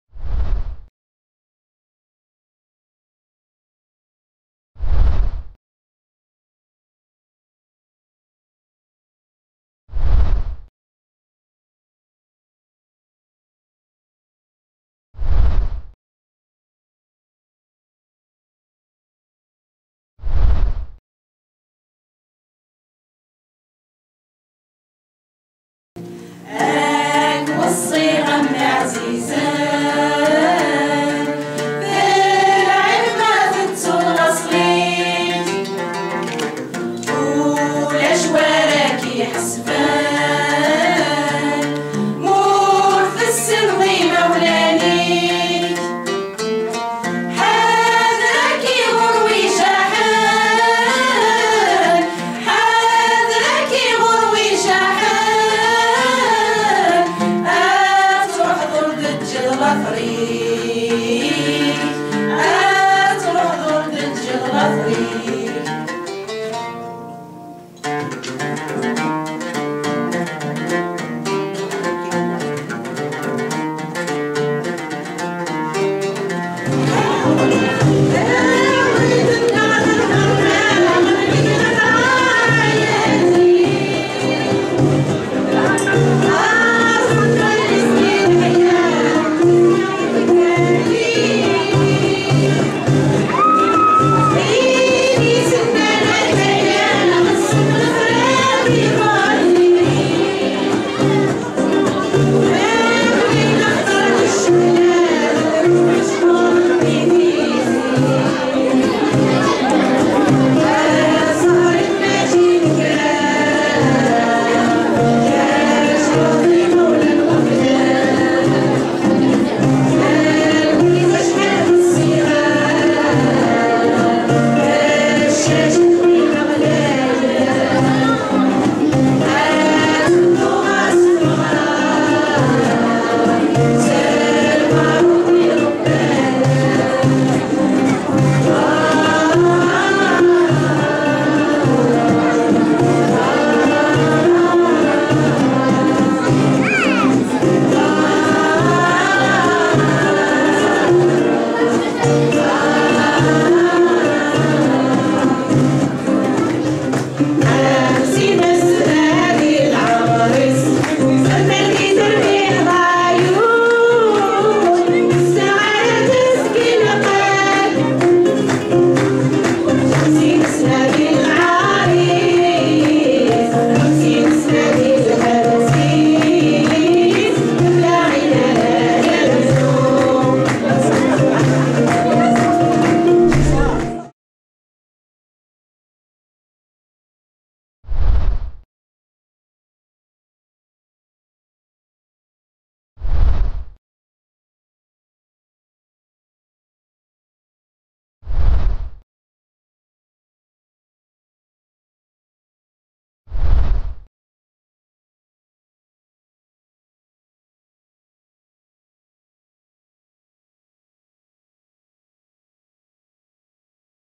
Troupe Tilleli Ottawa au spectacle de Zedek Mouloud à l'Olympia
Troupe Tilleli Ottawa au spectacle de Zedek Mouloud à l'Olympia de Montréal le Dimanche 04 Octobre 2015 à 15h